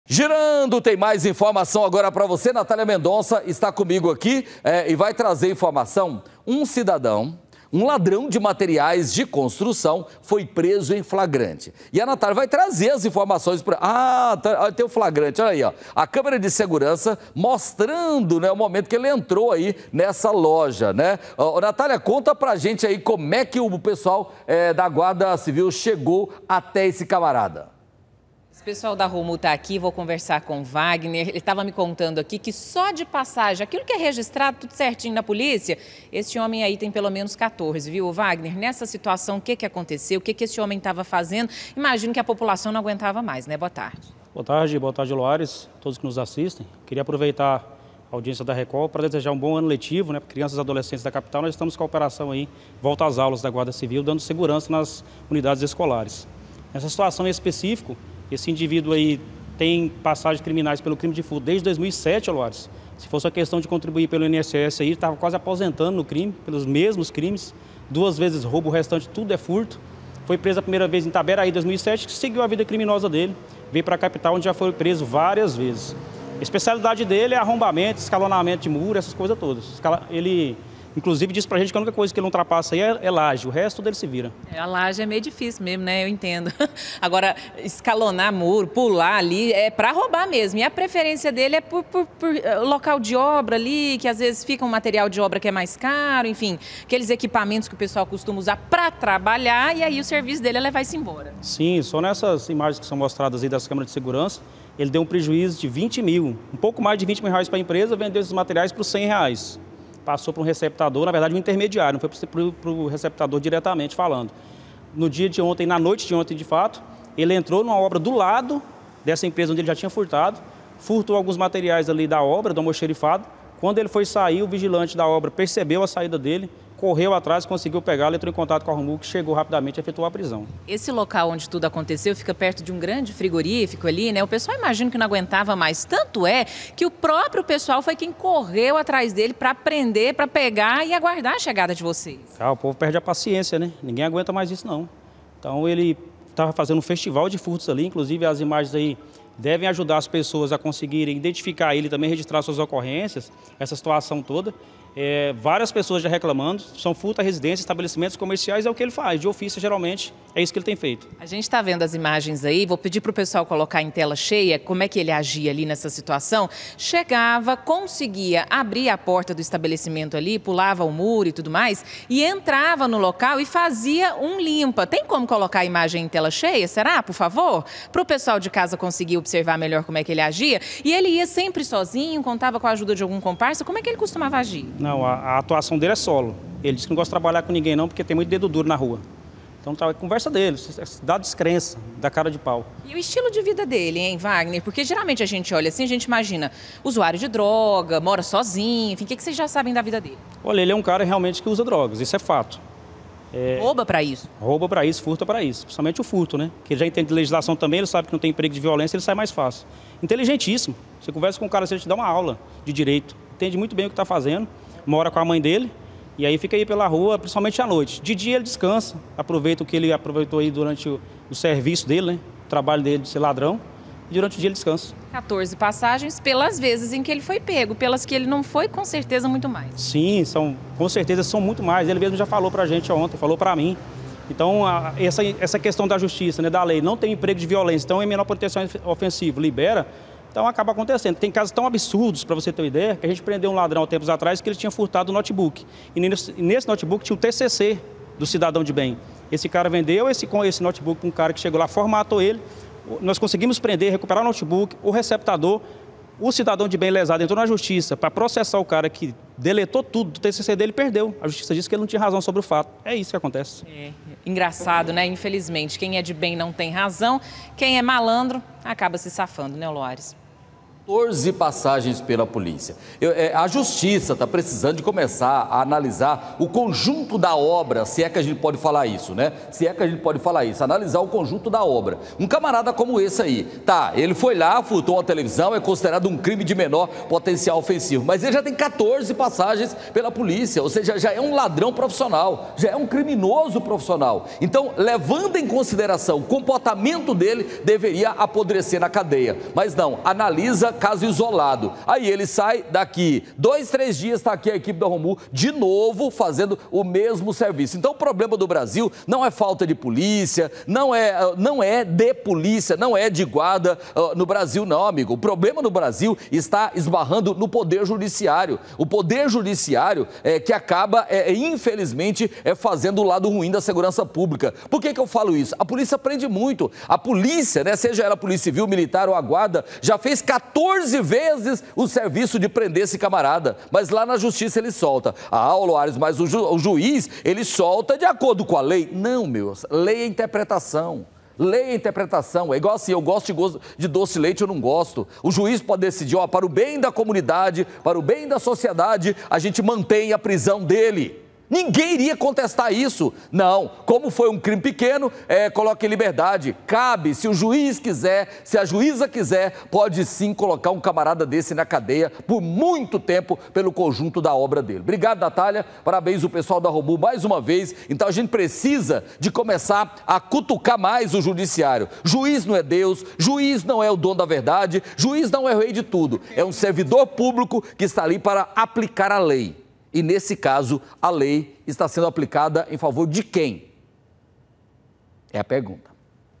A CASA CAIU PARA LADRÃO DE MATERIAIS DE CONSTRUÇÃO
LADRÃO DE MATERIAIS DE CONSTRUÇÃO É PRESO EM FLAGRANTE.